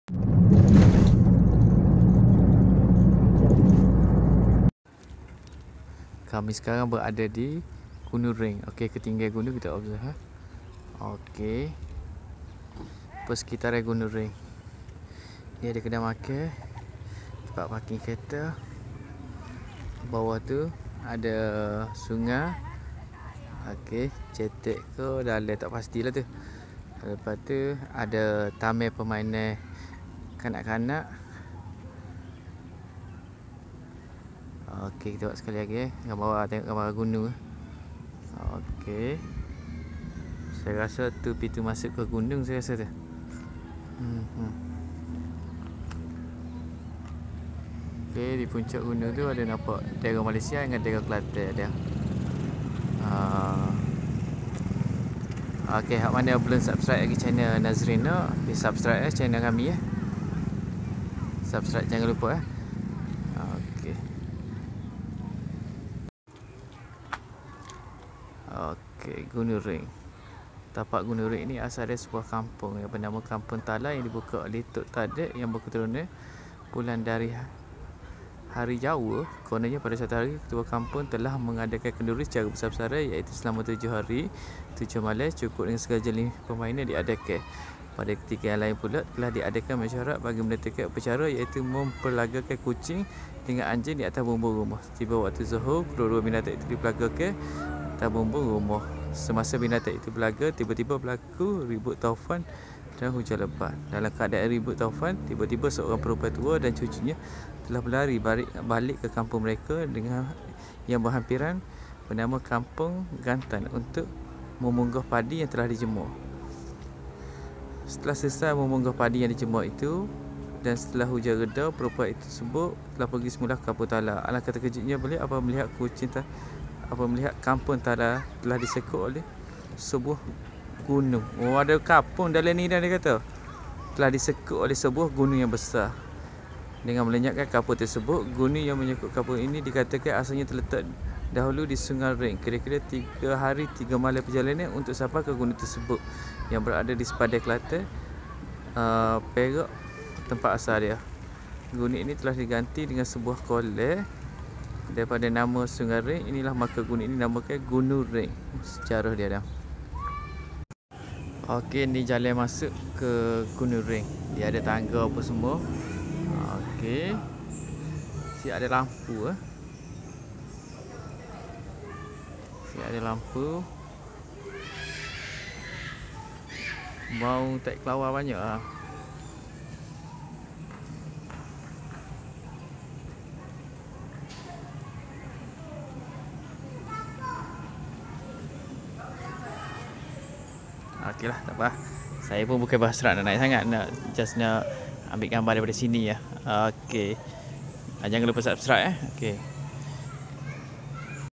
Gunung Reng Jeli Kelantan
Sebelum ni asyik lalu je, semalam 22/12/2019 baru terdetik nak singgah. Rupanya GunungReng tempat rekreasi popular daerah ni.

Pengunjung pun ramai.